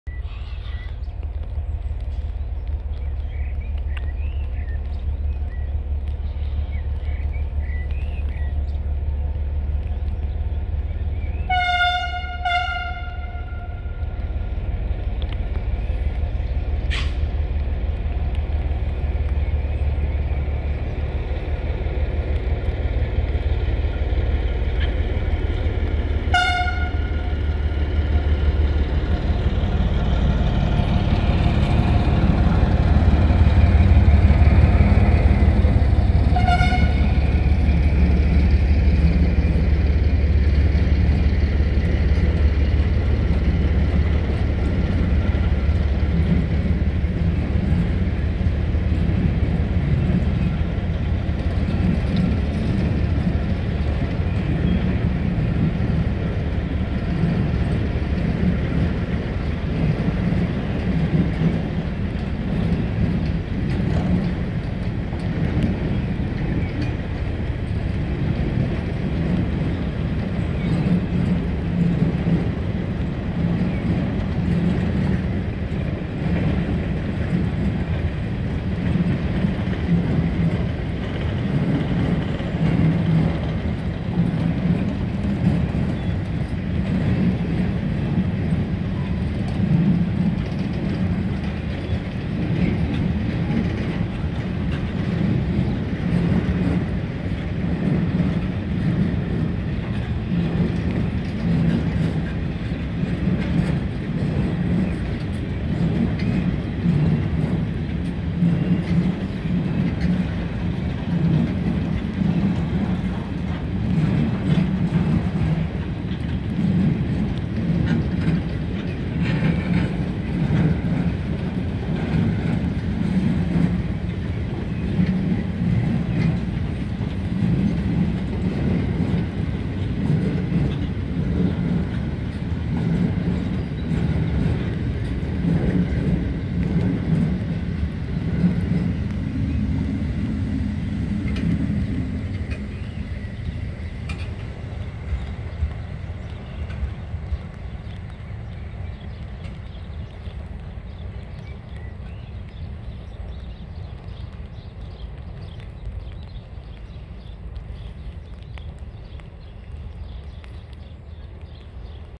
1 maart 2020
RFO 9702 komt met Lege BLG Autotrein en Achter RFO E-Loc 1837 van Leusden Pon Richting Amersfoort Centraal/Amersfoort Goederenemplacement Amersfoort.

Leusden,Lockhorsterbos